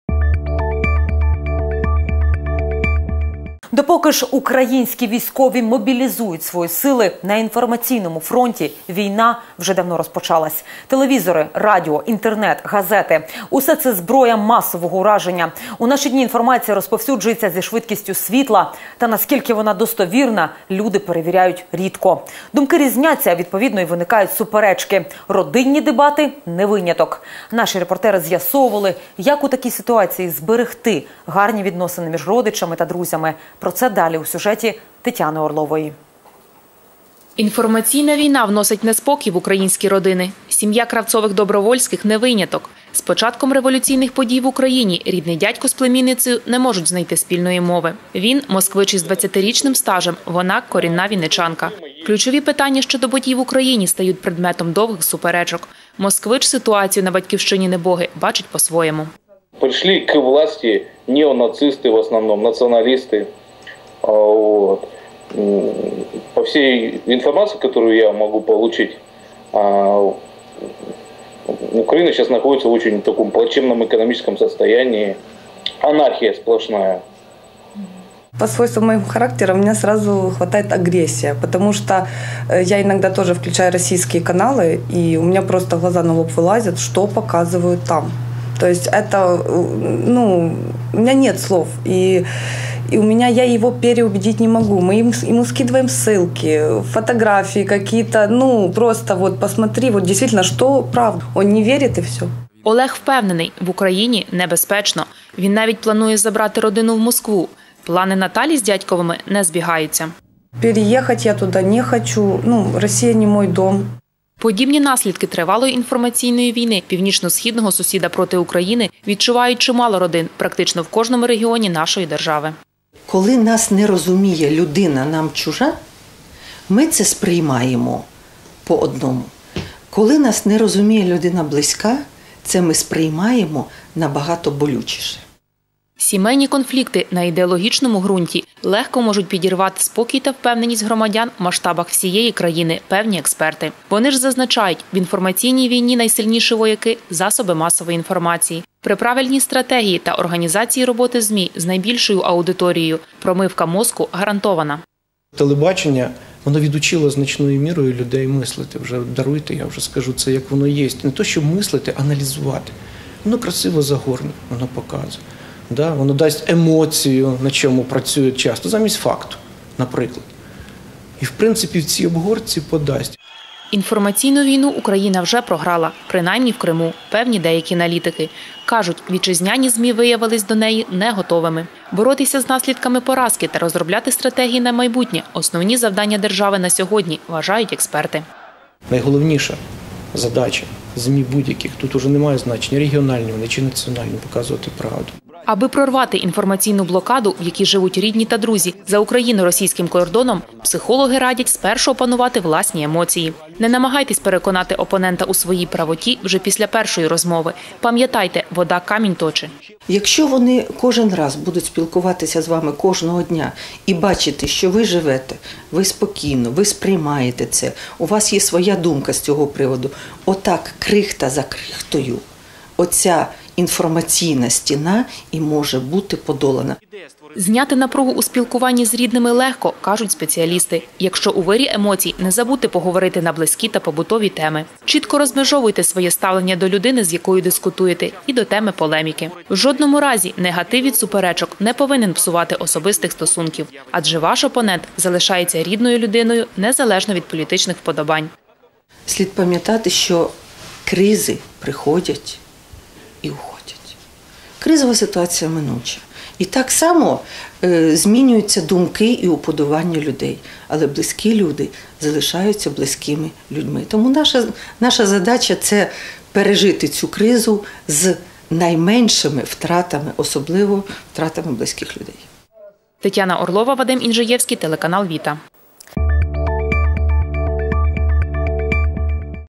Телеканал ВІТА новини 2014-03-18 Родинна і політика
Допоки ж українські військові мобілізують свої сили, на інформаційному фронті війна вже давно розпочалась. Телевізори, радіо, Інтернет, газети -- усе це зброя масового ураження. У наші дні інформація розповсюджується зі швидкістю світла. Та наскільки вона достовірна, люди рідко перевіряють. Думки різняться, а відповідно й виникають суперечки. Родинні дебати не виняток.